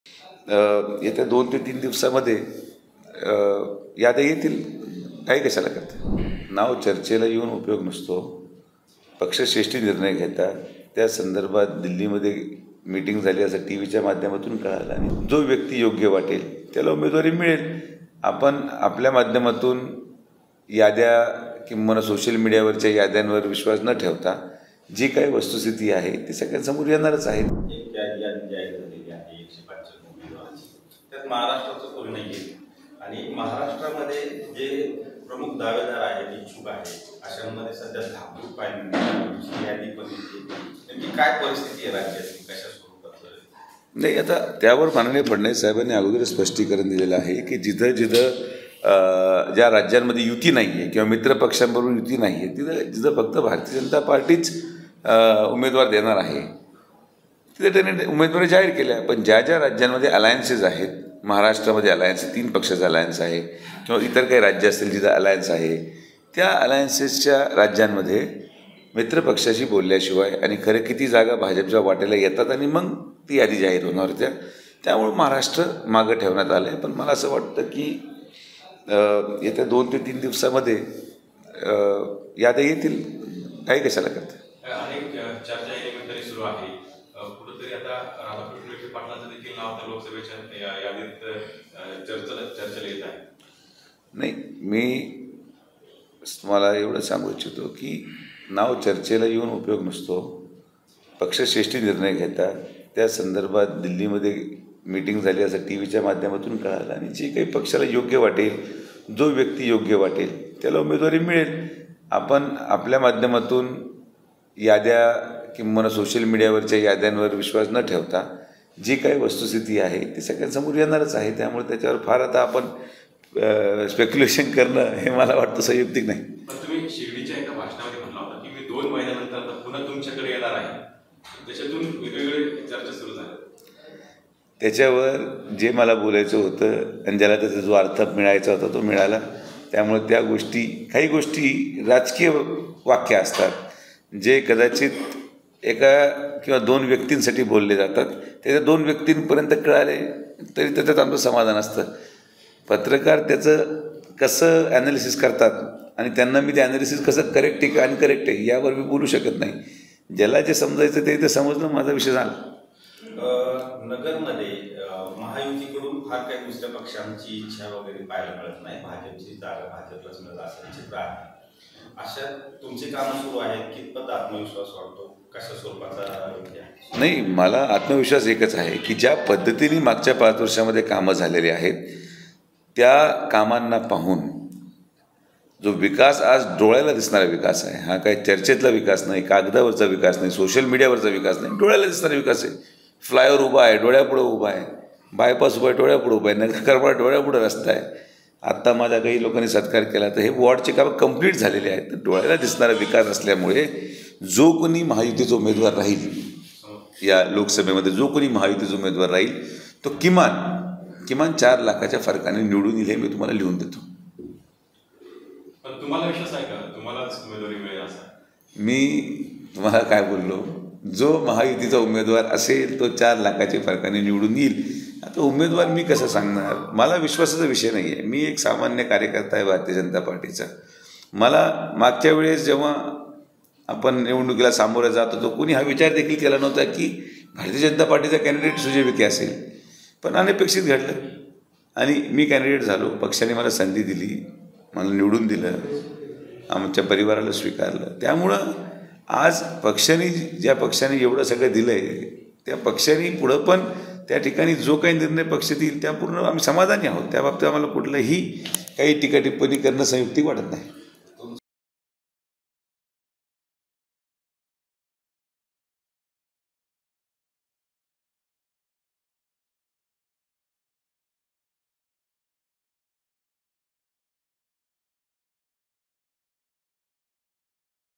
Sujay Vikhe Patil : वडिलांच्या लोकसभा उमेदवारीवर खासदार विखेंचे मोठे भाष्य | LetsUpp Marathi
आगामी लोकसभा निवडणुकीत राधाकृष्ण विखे पाटील भाजपाचे उमेदवार असतील का? या प्रश्नाचे सूचक उत्तर खासदार सुजय विखे यांनी दिले.